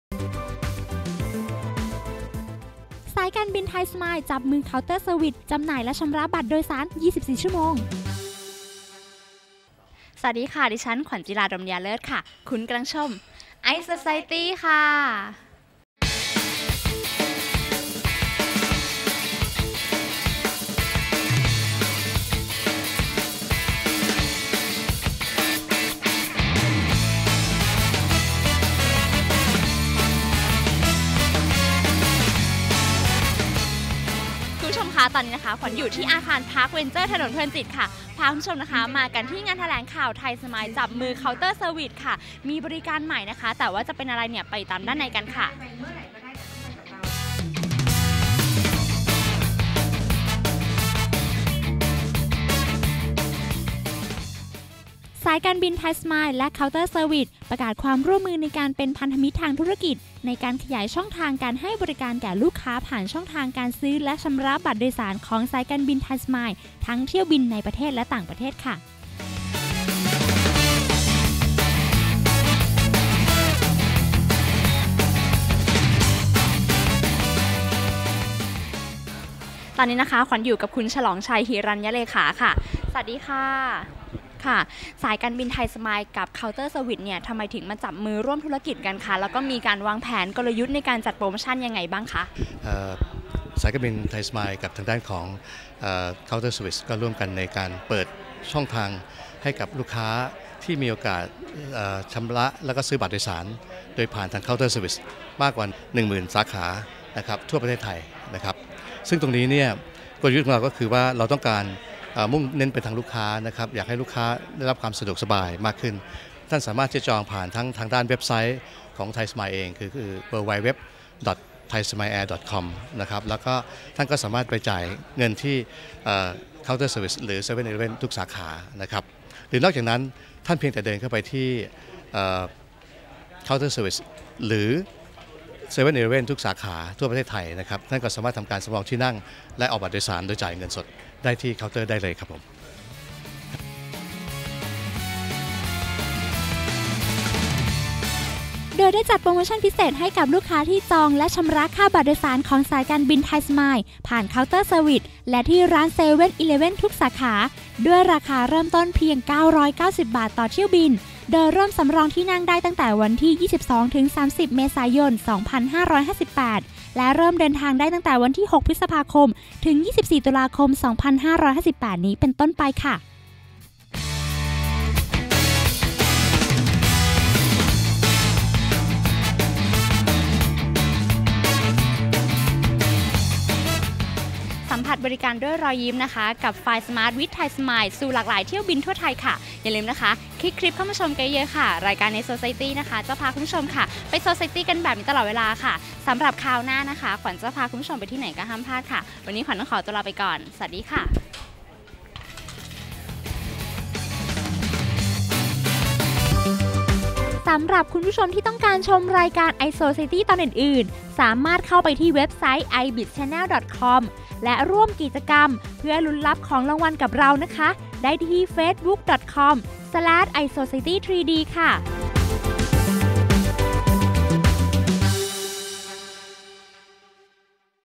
iSociety294 : ไทยสมายล์ จับมือ เคาน์เตอร์เซอร์วิส ให้บริการผ่านเคาน์เตอร์เซอร์วิส ทุกสาขาทั่วประเทศ
สายการบินไทยสมายล์ จับมือ เคาน์เตอร์เซอร์วิส จำหน่ายและชำระบัตรโดยสาร 24 ชั่วโมง

ติดตามชมรายการ iSociety และร่วมสนุกตอบคำถามรับของรางวัลได้ที่